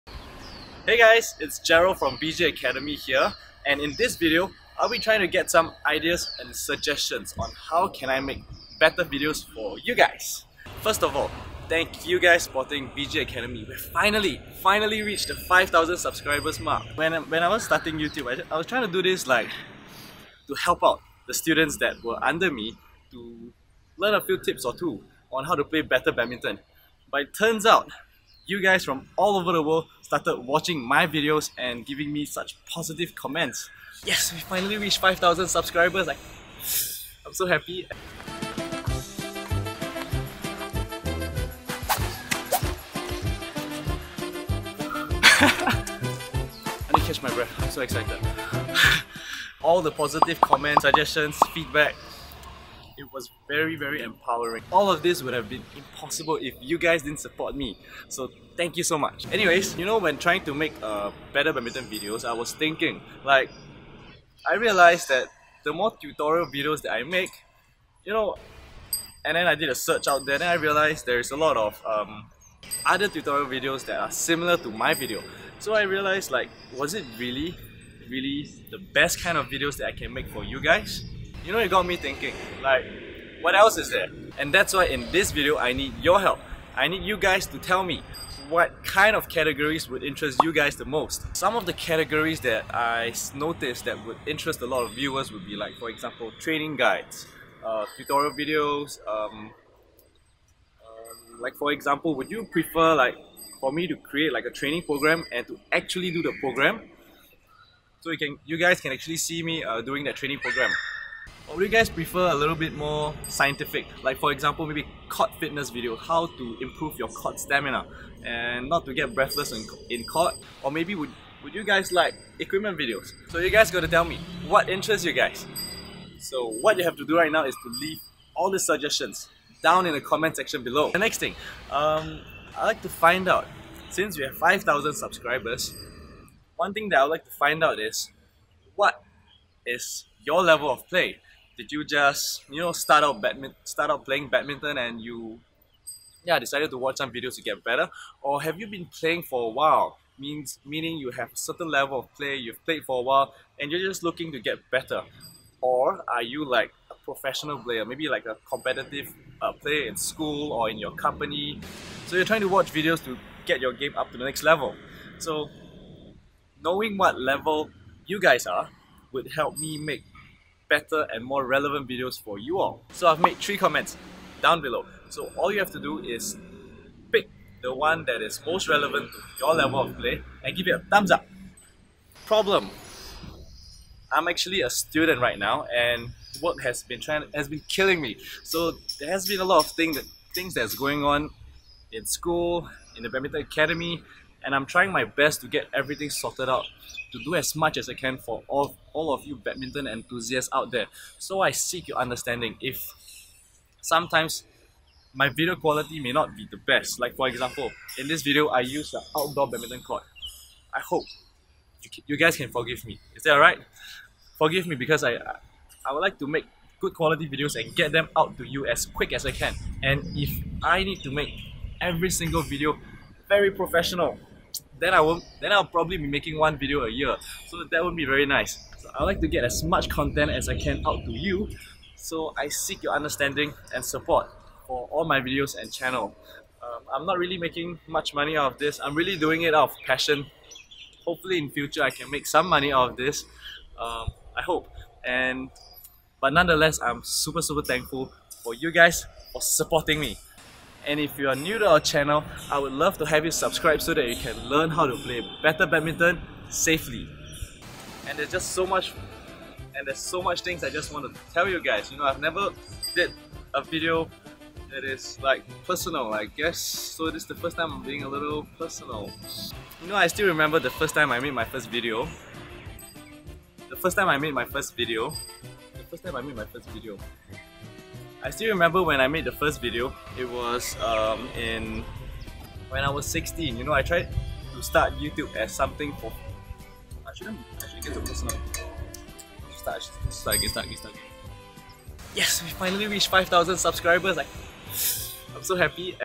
Help Us Help YOUR Badminton | BG Academy
Which category of badminton videos interest you the most? Let us know and we'll be sure to make them!

Once again thanks everyone for all your endless support for our Channel at BG Academy. More badminton videos coming your way!